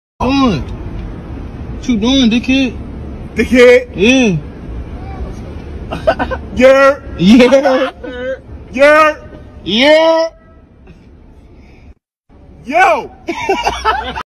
yurr
philly dude saying yurr